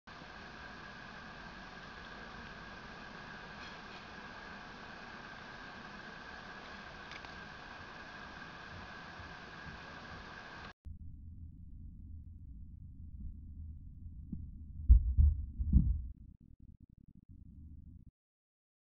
50 W Solarmodul Überproduktion! Statt 3.15 A über 4 A ! (Enormous sun intensity?!)
Eben bemerkt! Werner Altnickel in einer Wakenews-Sendung hatte Recht!
Wie ich das geschafft hab? Gar nicht, ich hab nur gemessen ;-)